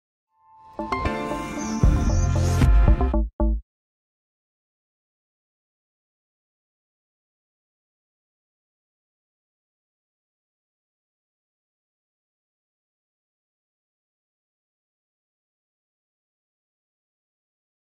תיעוד הקטינים חשודים בניסיון שוד בנק ברשל''צ קרדיט דוברות המשטרה
תיעוד קטינים חשודים בניסיון שוד בנק ברשל''צ קרדיט דוברות המשטרה The following video is a part of Haaretz news coverage.